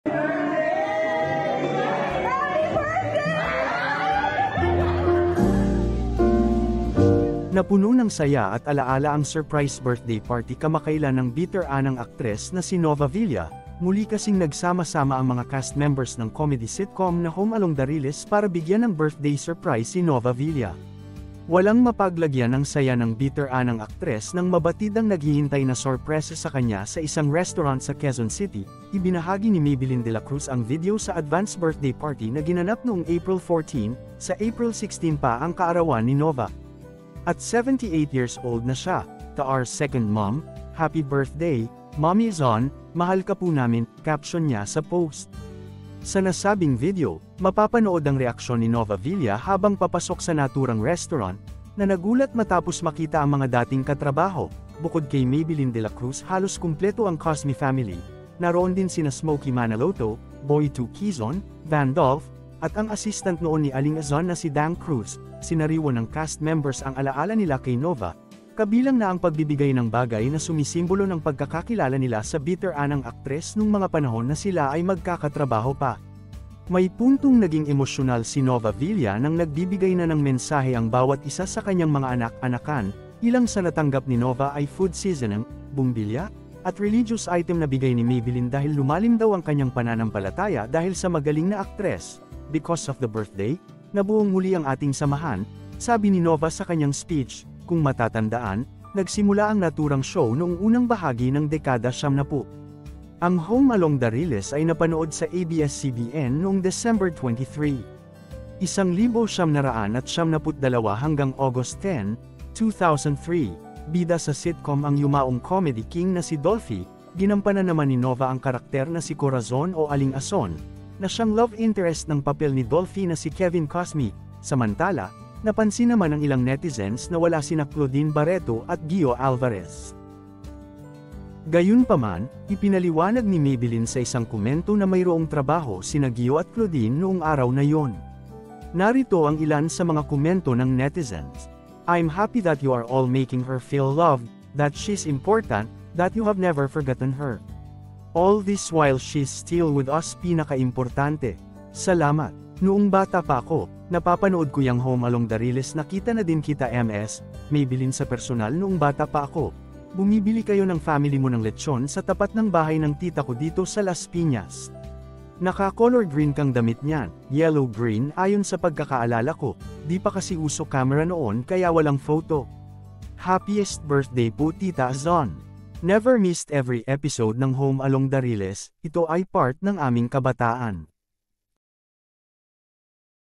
Nova Villa nakatanggap ng surprise birthday party mula sa casts ng Home Along Da Riles
Napuno ng saya at alaala ang surprise birthday party kamakailan ng beteranang aktres na si Nova Villa .Muli kasing nagsama-sama ang mga cast members ng comedy sitcom na Home Along Da Riles  para bigyan ng birthday surprise si Nova Villa.